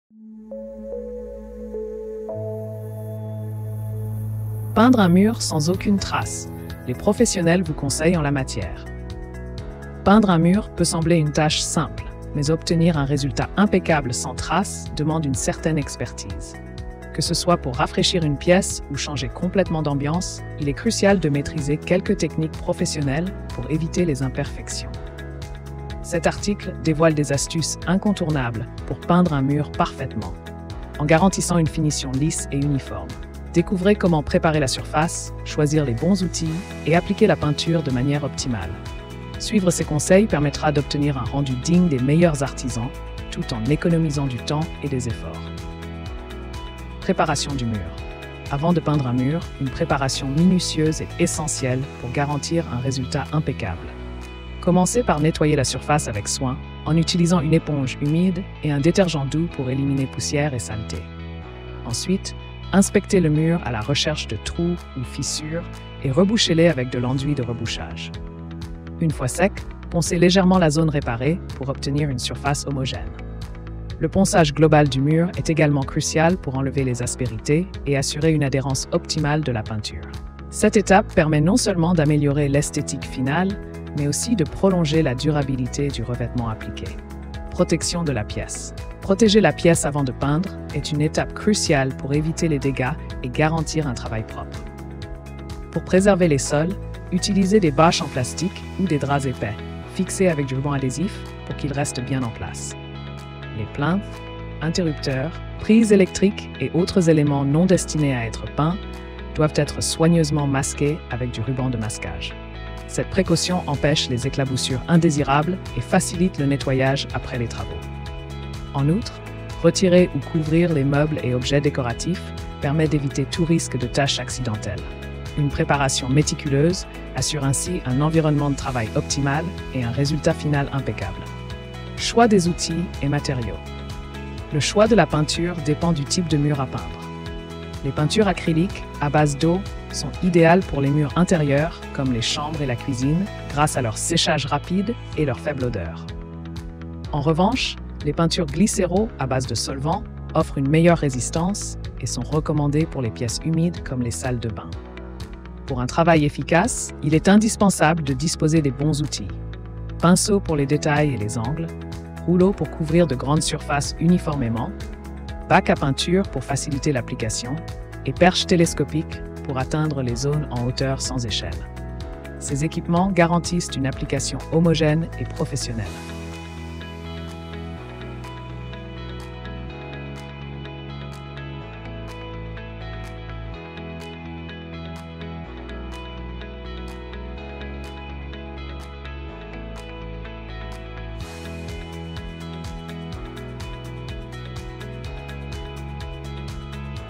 👉 Peindre un mur sans aucune trace : Les professionnels vous conseillent en la matière !
RDV sur notre site web 👉​

👉 Sommaire de la vidéo :
00:00 Peindre un mur sans aucune trace : Les professionnels vous conseillent en la matière !
00:47 Préparation du mur
01:28 Protection de la pièce
02:13 Choix des outils et matériaux